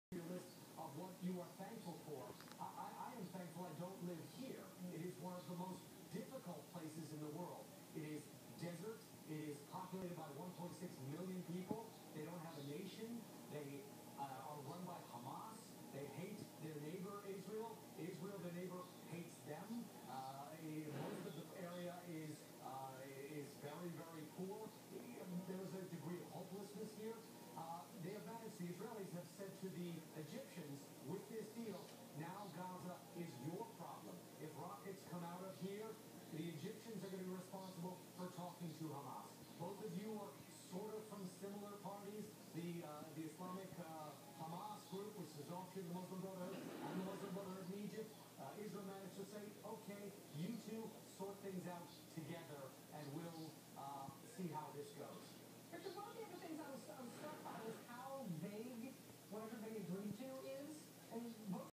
Dedi the dreaming roacher
Dedi has not only mastered roaching, she also dreams while doing it.